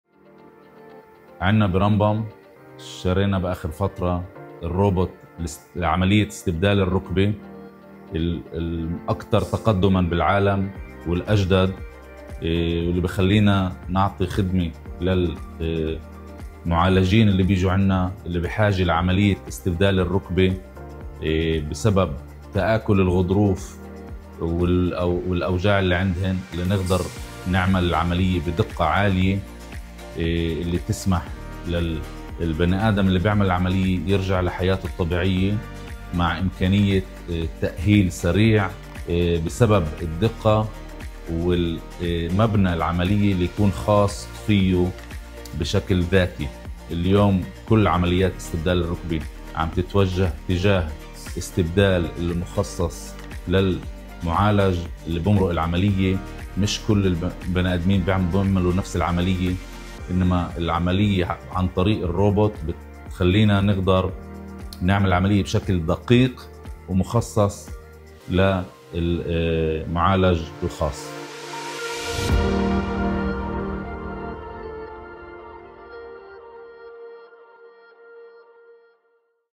خطوات أولى: الروبوت الأكثر تقدّمًا من نوعه يدخل الخدمة في رمبام لإجراء عمليّة استبدال مفصل الركبة
عندما يتعلّق الأمر بتوفير العلاج الطبيّ الأكثر تقدّمًا لعامّة الناس، فإنّنا نقصد ما يلي بالضبط: "يستحقّ عامّة الناس تلقّي أفضل التقنيّات المتوفّرة في المستشفيات الخاصّة، ولذلك فإنّ رمبام هو أوّل مستشفى عامّ في الشمال يتبنّى أحدث التقنيّات لإجراء عمليّات استبدال مفصل الركبة"، يقول د. نبيل غريّب.

قام مستشفى رمبام، مؤخّرًا، باقتناء الروبوت الأكثر تقدّمًا من نوعه في العالم لإجراء عمليّة استبدال مفصل الركبة، لوحدة جراحة العظام في مستشفى رمبام، وبذلك يصبح رمبام المستشفى الوحيد في منطقة الشمال الذي يجري هذه العمليّة الجراحيّة كمستشفى عامّ. وتسعى وحدة جراحة العظام، من خلال هذا الروبوت، إلى تزويد سكّان شمال دولة إسرائيل بقمّة القدرات التكنولوجيّة في هذا النوع من العمليّات الجراحيّة، مع توسيع سلّة الخدمات الجراحيّة المقدّمة للجمهور، بمعايير طبّيّة رائدة.

لمَن تُجرى العمليّة الجراحيّة لاستبدال مفصل الركبة؟
يسمح الروبوت لوحدة جراحة العظام في رمبام بتوسيع سلّة الخدمات المقدّمة لجمهور المعالَجين، من خلال تطوير أساليب جراحيّة أكثر تقدّمًا، وهو ما يُمكِّن المعالَجين الذين يعانون من تآكل الغضاريف وإصابات في المفاصل وغيرها من الحصول على علاج دقيق وملاءَم بشكل شخصيّ لمشكلة العظام الموجودة في الركبة، الأمر الذي له أبعاد مستقبليّة، سواء على عمليّة إعادة التأهيل أو على جودة الحياة في فترة ما بعد إعادة التأهيل.
ويختتم د. نبيل غريب، نائب مدير وحدة جراحة العظام، المسؤول عن مجال استبدال المفاصل في الوحدة، قائلًا: "إنّه روبوت متطوّر للغاية، يتمتّع بدقّة عالية جدًّا جدًّا، ويتيح لنا، كجرّاحين، إجراء العمليّات الجراحيّة بمستوى أعلى من الجودة، بل وبشكل أسهل طوال فترة العمليّة الجراحيّة. إنّ وجوده يسمح لنا بإجراء عمليّة جراحيّة ملاءَمة بشكل شخصيّ لكلّ معالَج، وبتحديد المشكلة التي يعاني منها والتي يخضع بسببها لهذا الإجراء الطبيّ. اليوم، مع إدخال الروبوت ضمن الخدمة، فإنّ مستشفى رمبام ينضمّ إلى المراكز الطبّيّة الرائدة في العالم، ويتيح لجمهور مُعالَجيه اختيار تلقّي نفس الخدمة لكن في مستشفى عامّ يتضمّن جعبة واسعة من المزايا، تتمثّل في الخبرة، التقدّم والخدمة الراقية."